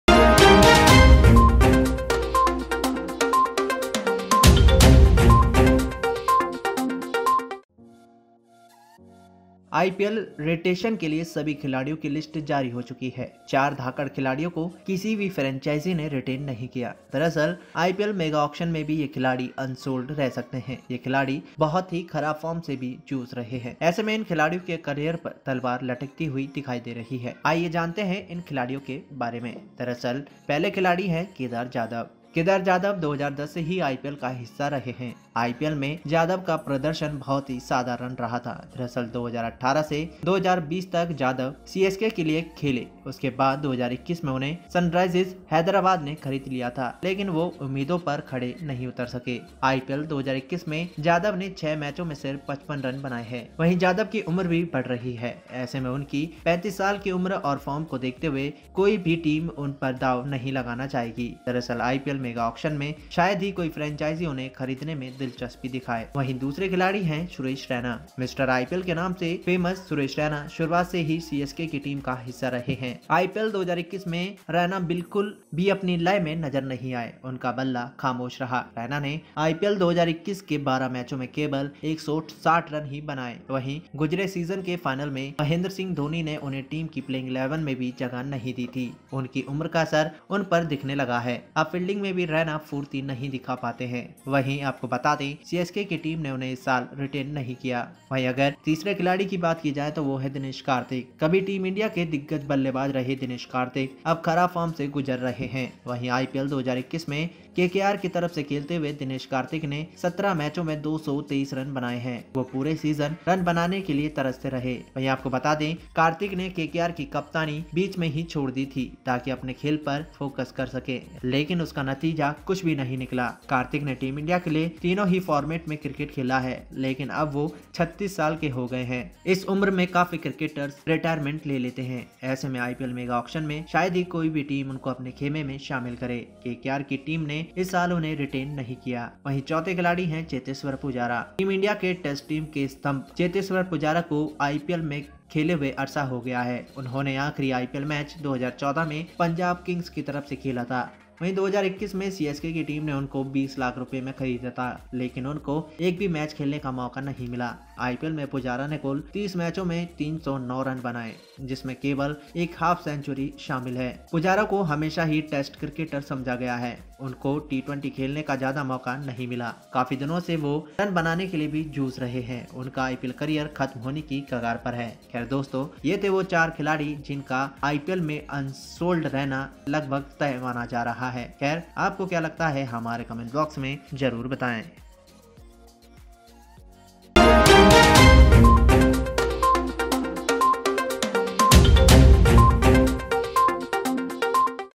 IPL 2022 Mega Auction में अनसोल्ड रहेंगे ये धाकड़ खिलाड़ी! खत्म होने के कगार पर करियर!
HEADLINE :IPL 2022 Mega Auction में अनसोल्ड रहेंगे ये धाकड़ खिलाड़ी! खत्म होने के कगार पर करियर!